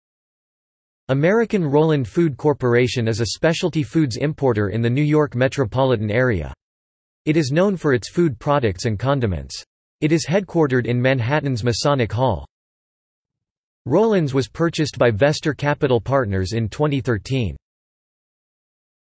Roland Foods Corporation | Wikipedia audio article
This is an audio version of the Wikipedia Article:
Roland Foods Corporation

- Socrates

SUMMARY
American Roland Food Corporation is a specialty foods importer in the New York metropolitan area. It is known for its food products and condiments. It is headquartered in Manhattan's Masonic Hall.
Roland's was purchased by Vestar Capital Partners in 2013.